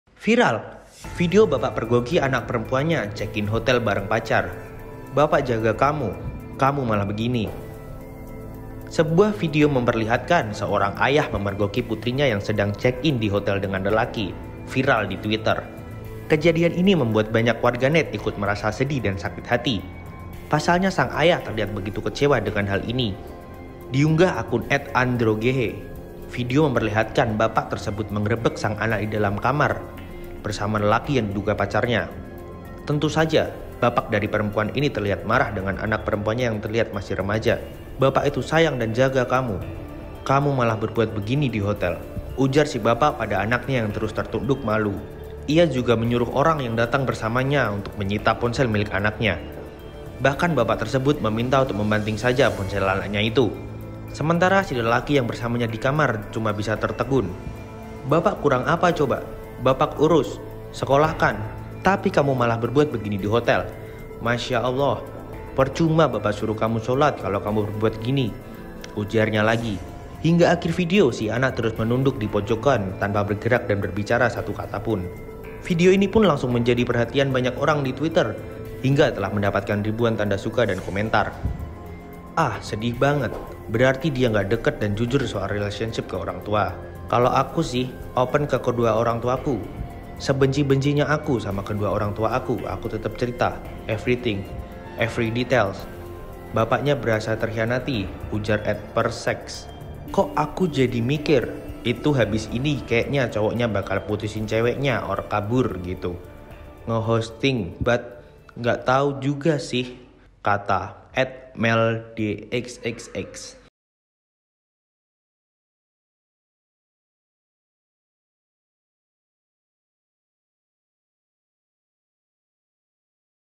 Viral Video Bapak Pergoki Anak Perempuannya Check In Hotel Bareng Pacar: Bapak Jaga Kamu, Malah Kaya
Viral Video Bapak Pergoki Anak Perempuannya Check In Hotel Bareng Pacar: Bapak Jaga Kamu, Malah Kaya Gini

Sebuah video memperlihatkan seorang ayah memergoki putrinya yang sedang check in di hotel dengan lelaki, viral di Twitter. Kejadian ini membuat banyak warganet ikut merasa sedih dan sakit hati, pasalnya sang ayah terlihat begitu kecewa dengan hal ini.

Diunggah akun @Androngehe, video memperlihatkan bapak tersebut menggerebek sang anak di dalam kamar hotel bersama lelaki yang diduga pacarnya. Tentu saja, bapak dari perempuan itu terlihat marah dengan anak perempuannya yang terlihat masih remaja.

"Bapak itu sayang dan jaga kamu, kamu malah berbuat begini di hotel," ujar si bapak pada anaknya yang terus tertunduk malu.

Vo/Editor : Didan/Marcell